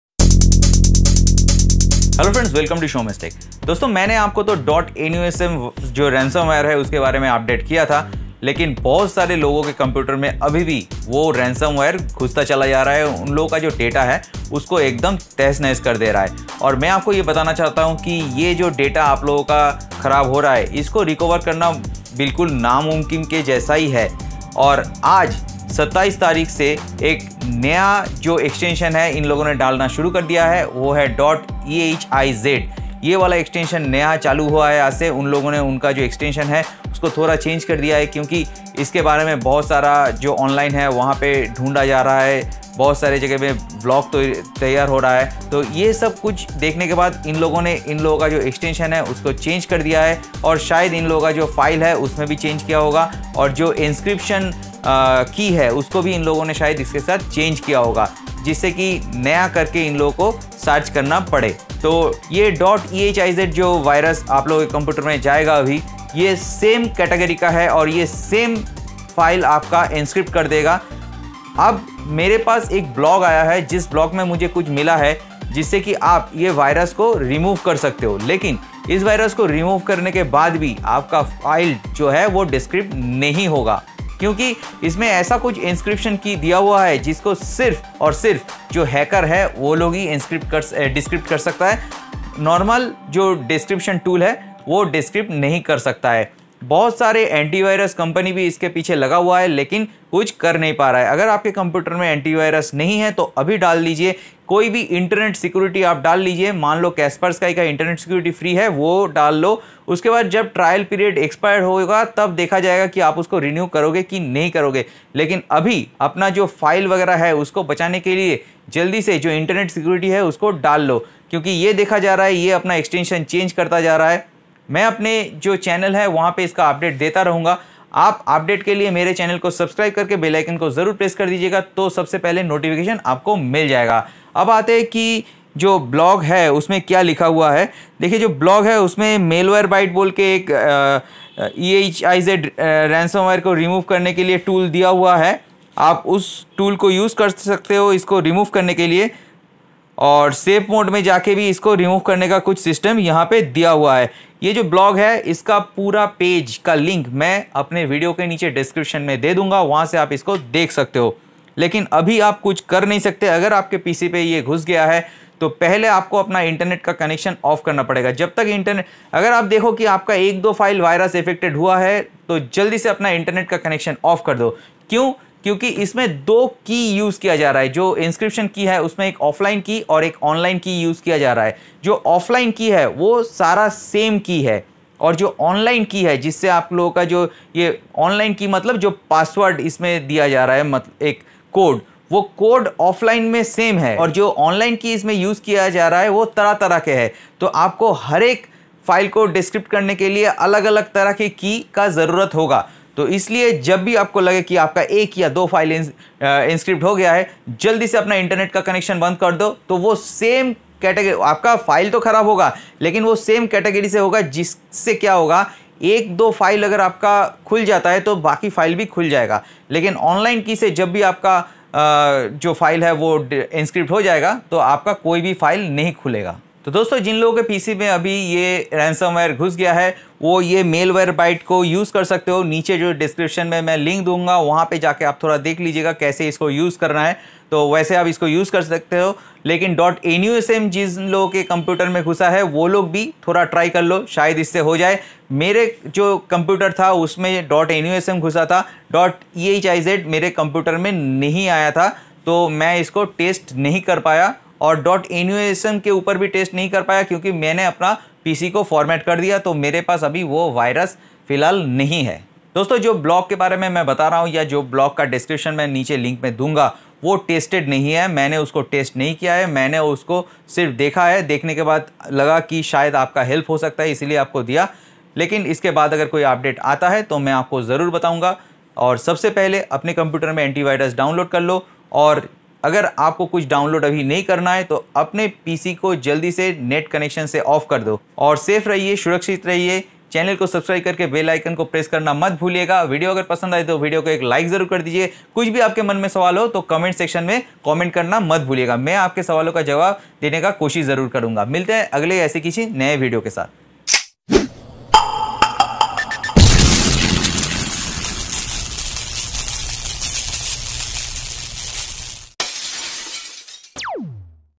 New 2021 ehiz Computer Virus Update 💥 .ehiz Ransomware 💥 How to remove
New 2021 ehiz Computer Virus Update.
 How to remove .ehiz Ransomware from computer.